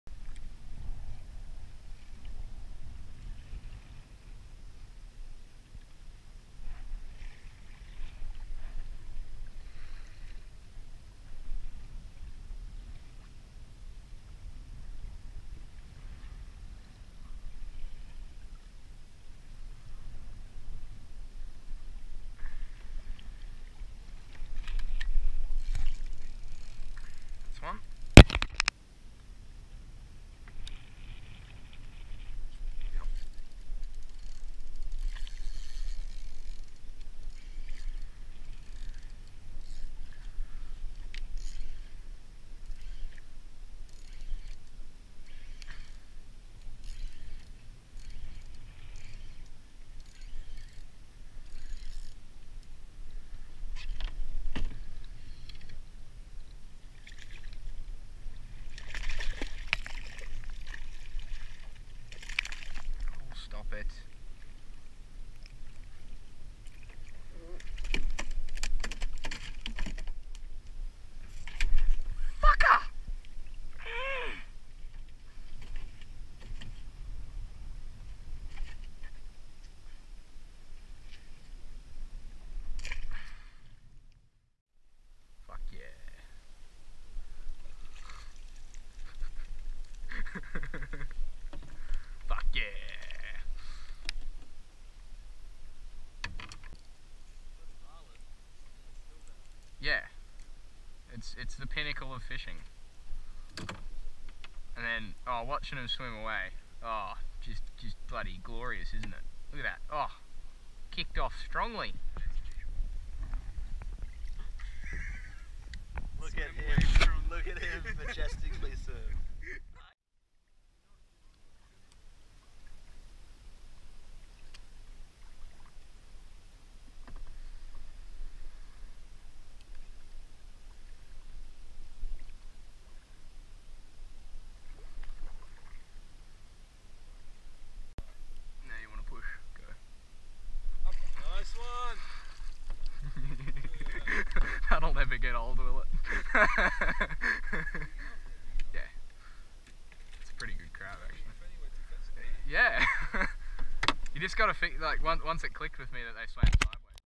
Weekend fishing- snapper, grinners and crabs
Good weekend rustling up a feed- got one decent snapper (45cm) which I turned the camera off just in time for, as well as an undersized model and a few grinners. Caught a few free-swimming crabs just with the landing net too!